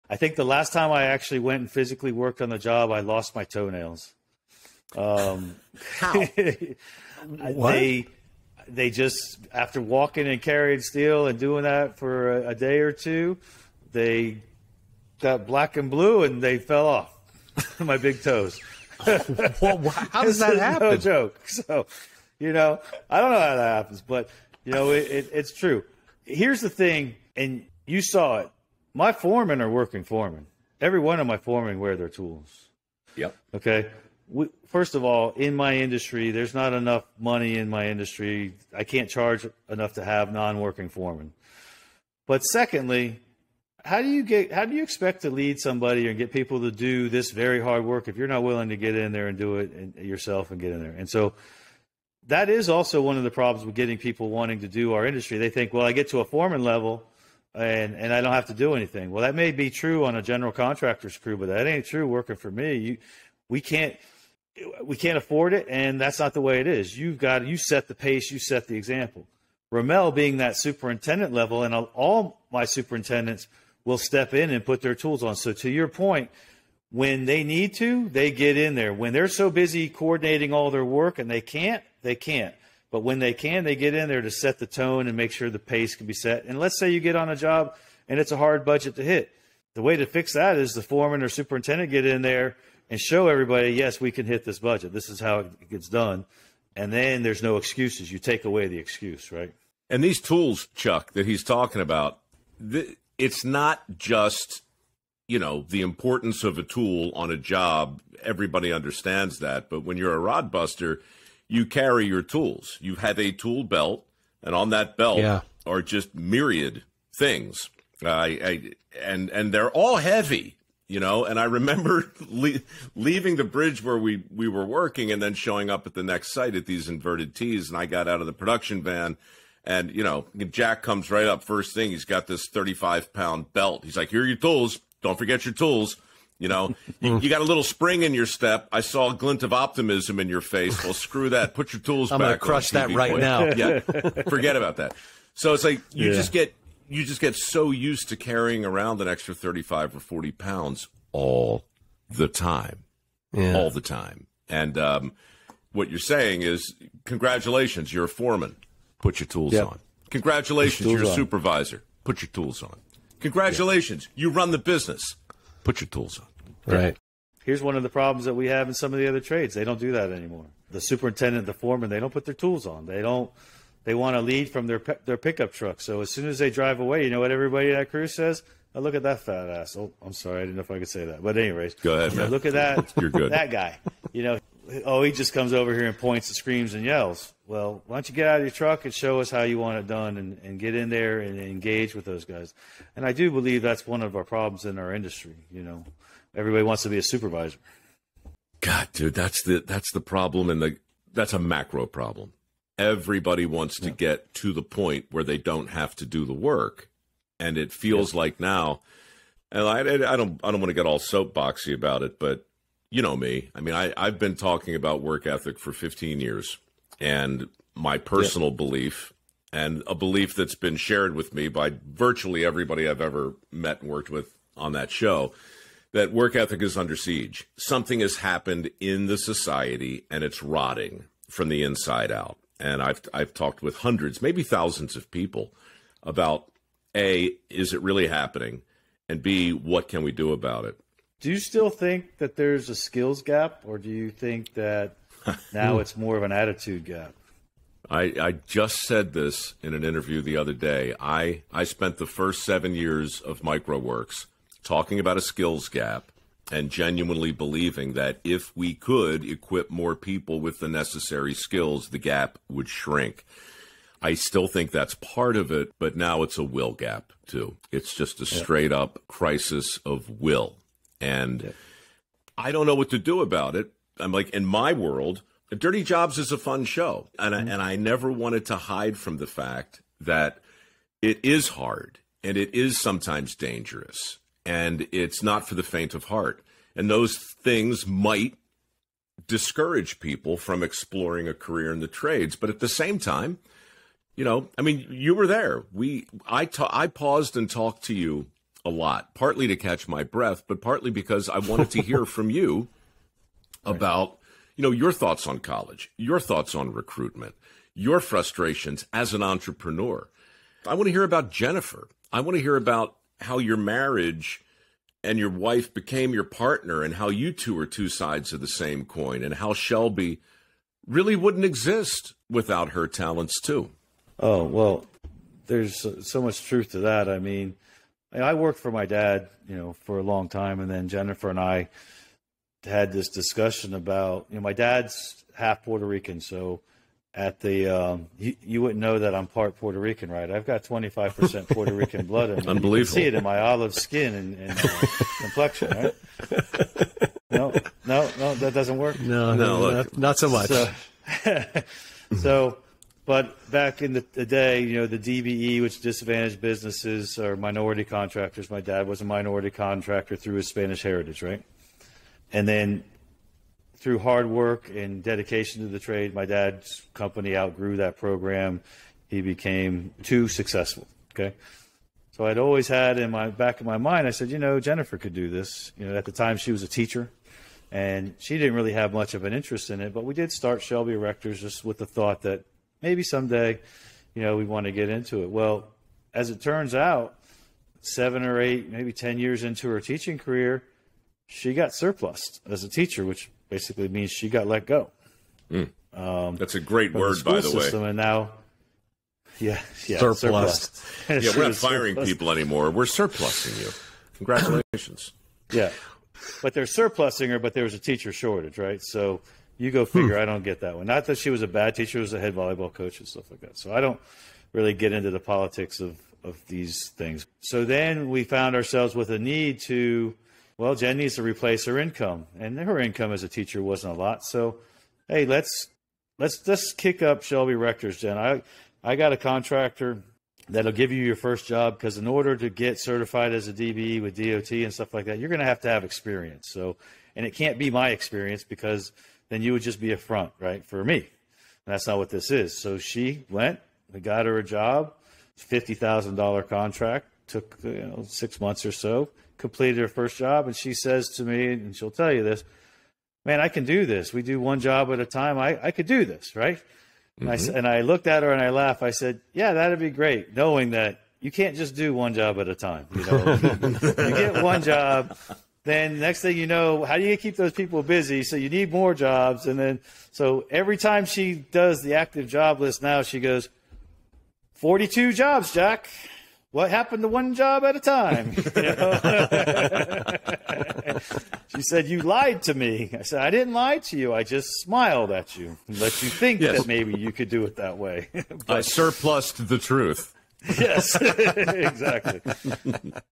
You Can’t Lead from a Pickup Truck (Ep. 283)
Forget about politics for a moment, if you can. Forget about labor vs. management and union vs. non-union if you can. Just think about work as you listen to Jack Nix. And as you listen, ask yourself if the country might learn something from this man.

I met Jack on Dirty Jobs over a year ago. He and his wife have a company called Shelby Erectors, Inc.. Jack is an entrepreneur. He's also an ironworker, through and through. He builds bridges down in Florida. Sometimes, he's got a hundred guys on the payroll. Sometimes, two-hundred.

Jack and his merry band of rodbusters kicked my ass, but good, on the first segment of the Dirty Jobs reboot. And during that shoot, he said a lot of things that made me wish I had a long form podcast, so I could invite him on as a guest one day, to have a conversation about the importance of carrying your own tools, and the abject futility of attempting to lead from a pick-up truck.

That box, is now checked.